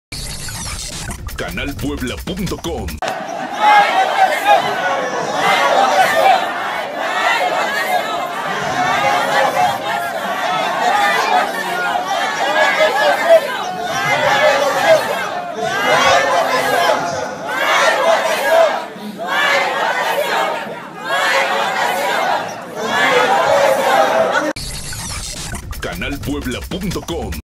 San Miguel Canoa Elecciones
Pobladores de Canoa impiden instalación de casillas